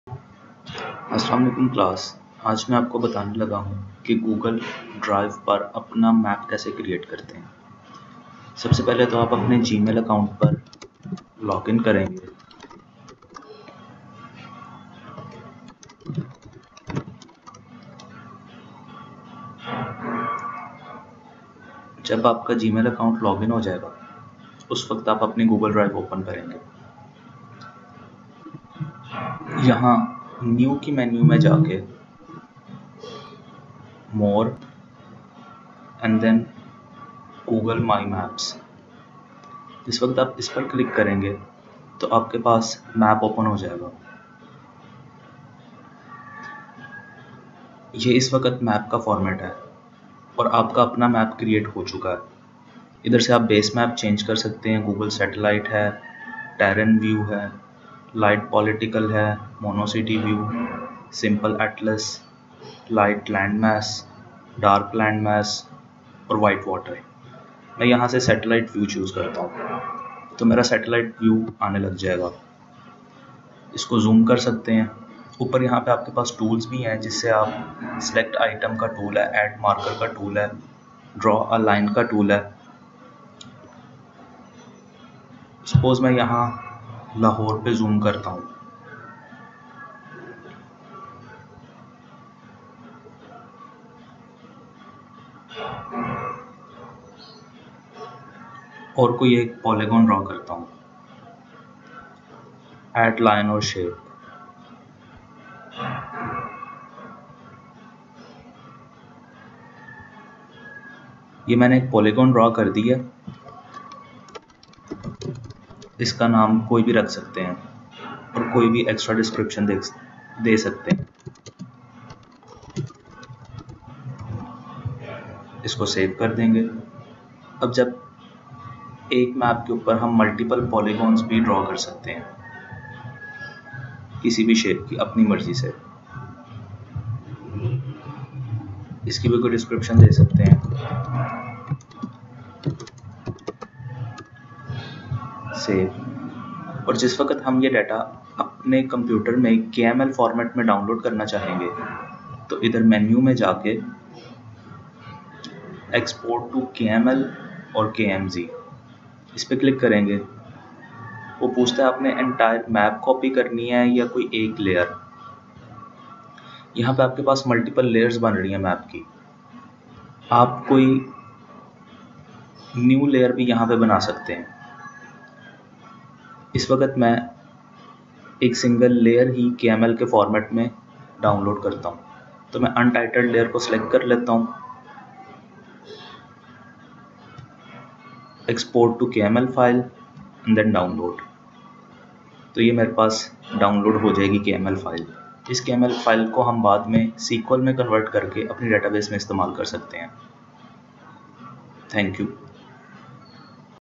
Create a Google map on Google Drive to use with any Database
this is a simple map creation tutorial.